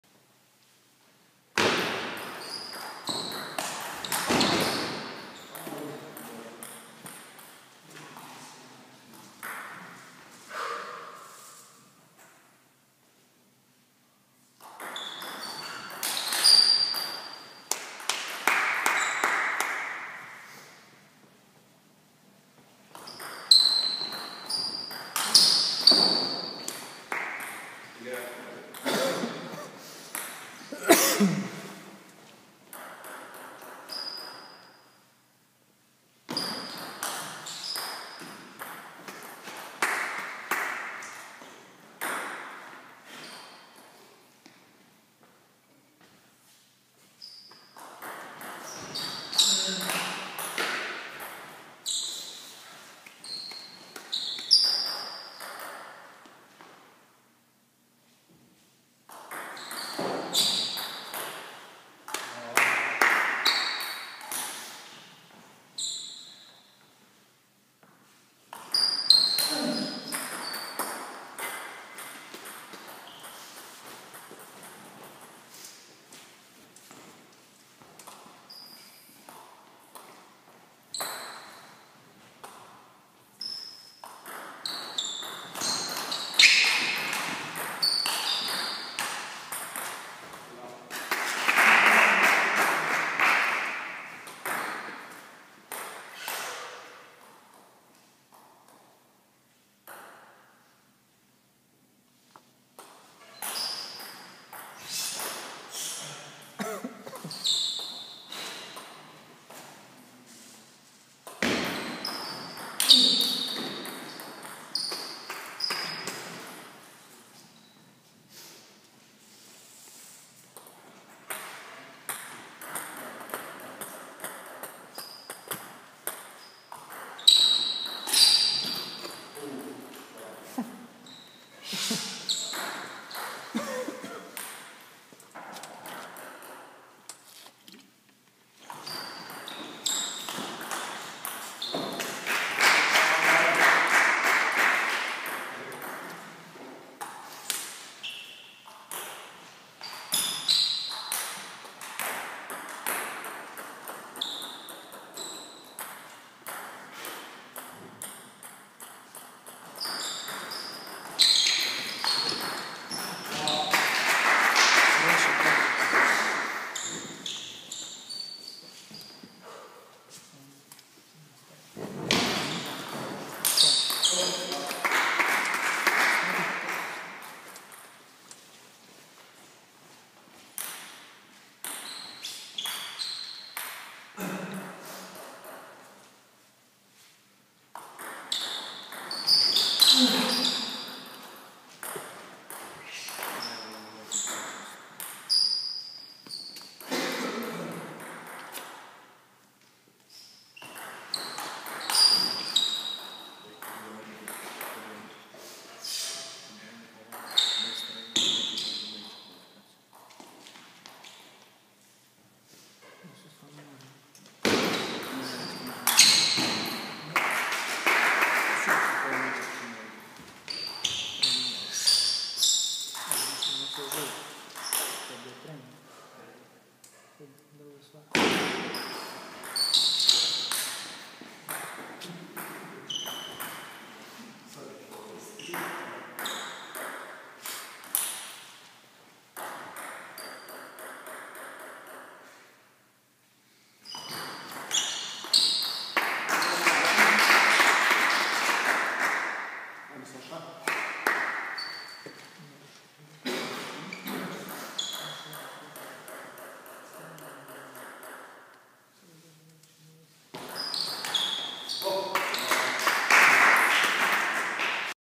Kinkela Daniel - Stankovski Saša 2. set
Druga hrvatska stolnoteniska liga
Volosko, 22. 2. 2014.
Opatija 08 - Brovinje 1 : 4
Kinkela Daniel - Stankovski Saša 1 : 3 (-9, 9, -8, -5)